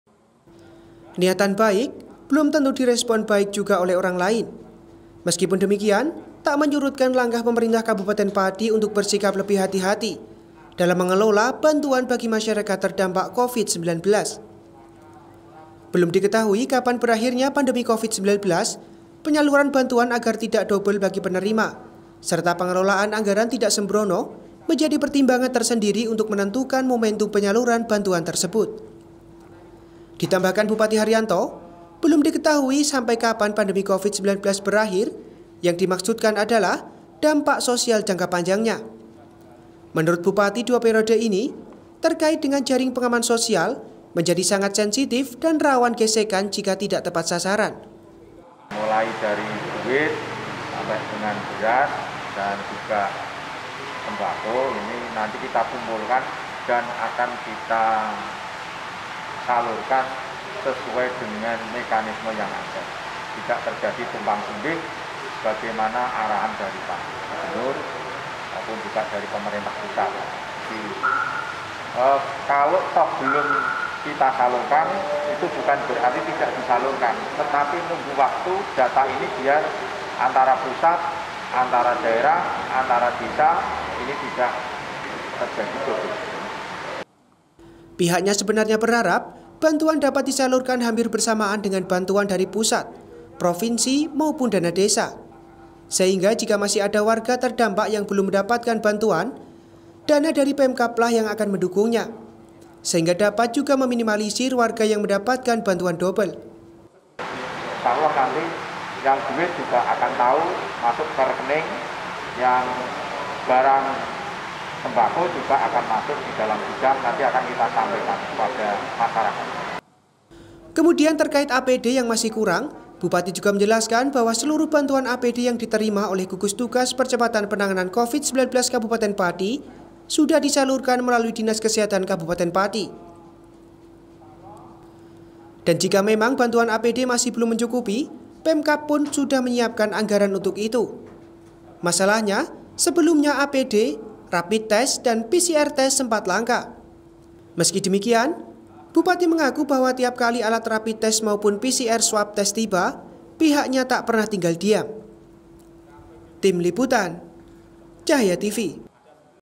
Bulan Mei 2020, Bantuan Pemkab Pati Di Salurkan
Masyarakat Pati Terdampak Covid 19 Diminta Untuk Lebih Bersabar, Terkait Dengan Penyaluran Bantuan Dari Pemkab Setempat. Ada Sejumlah Catatan, Membuat Bupati Haryanto Memilih Bulan Mei Bantuan Baru Di Luncurkan.